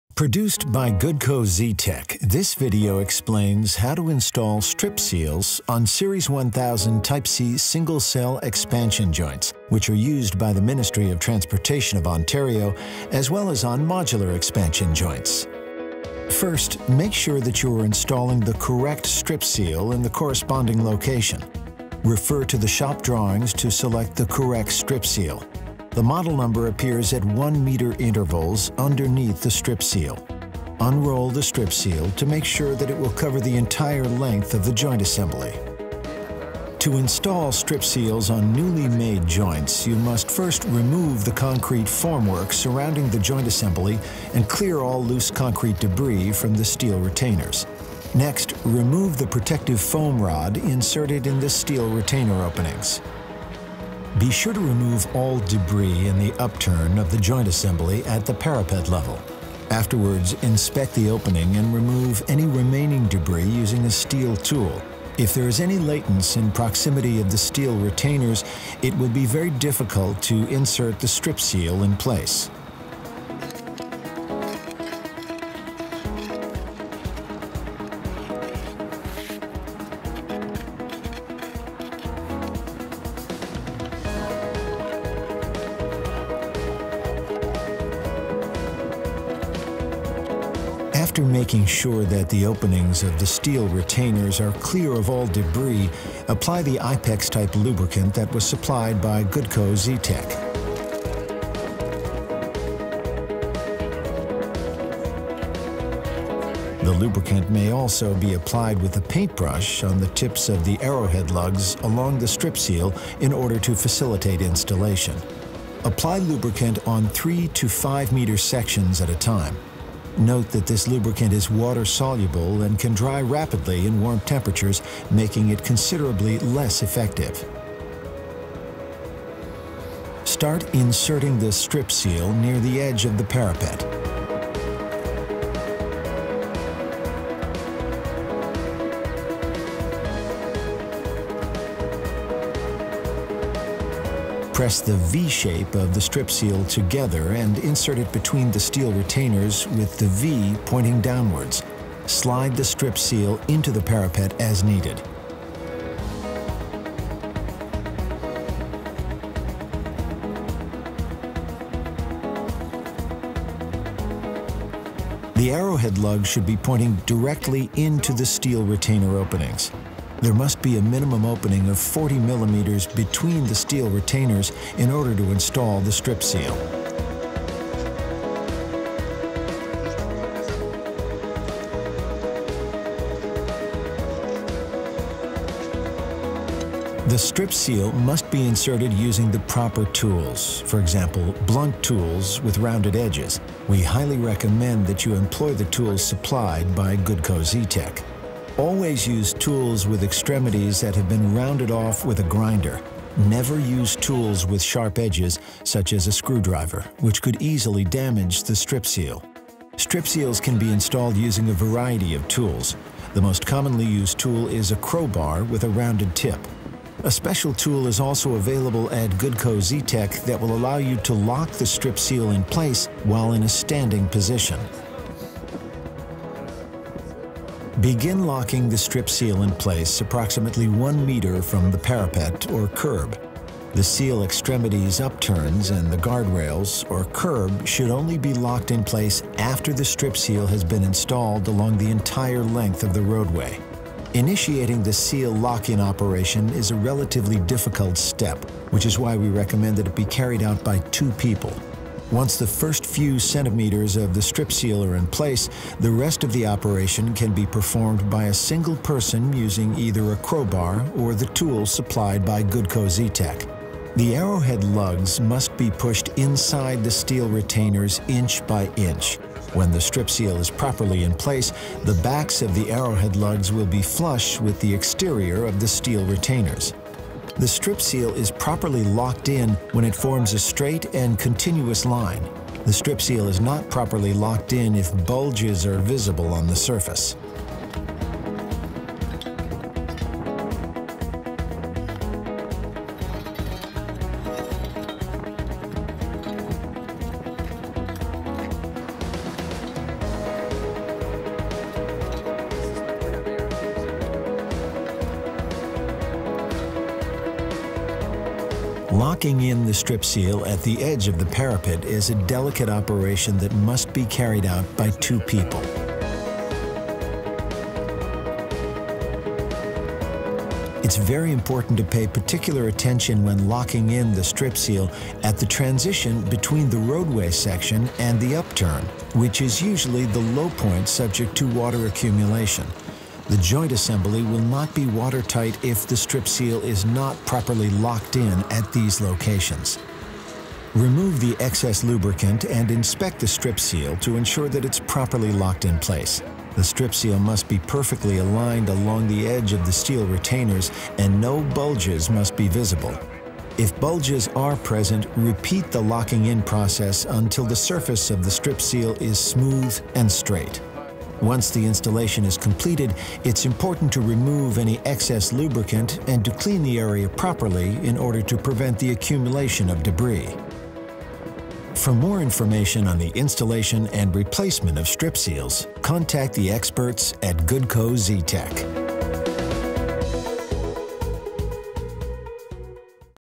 How to install strip seal expansion joints
Produced by Goodco Z-Tech, this video explains how to install strip seals on Series 1000, Type C single cell expansion joints, which are used by the Ministry of Transportation of Ontario, as well as on modular expansion joints.

For more information on the installation and replacement of strip seals, contact the experts at Goodco Z-Tech at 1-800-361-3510.